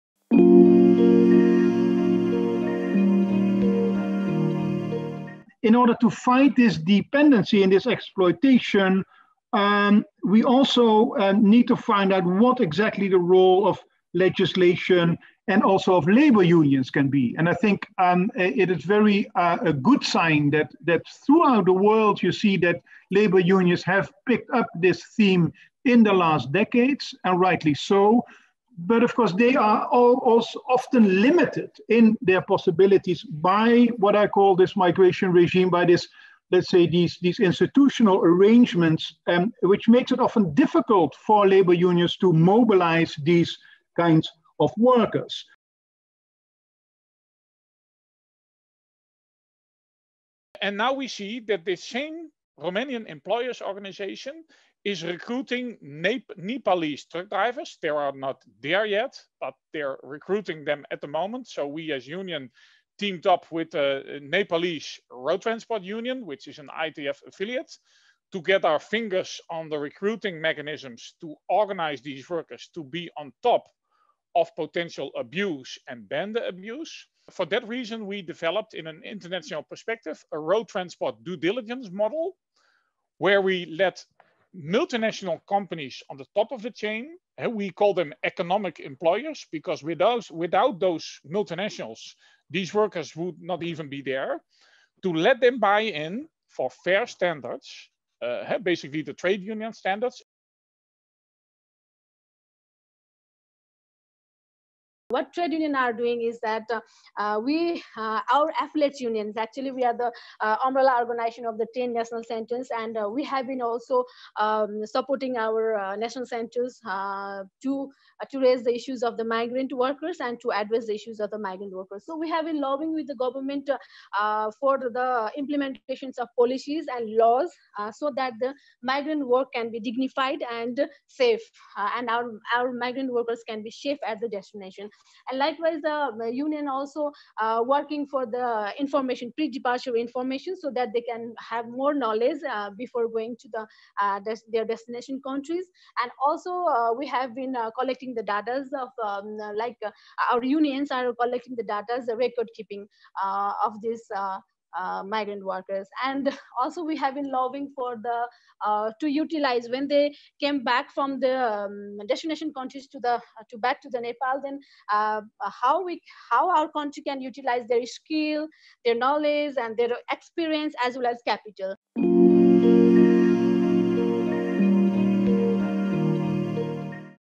Webinar Migratie - Deel 5: What can trade unions do?
Deel 5 - De omstandigheden waaronder arbeidsmigranten vanuit Zuid-Azië naar Europa migreren zijn zorgelijk en moeten beter. Om de dynamiek erachter te begrijpen en kennis hierover te delen brachten we een internationaal gezelschap van wetenschappers, vakbonden, politici en ngo’s bij elkaar. Een interessant inkijkje in de wereld van de arbeidsmigranten.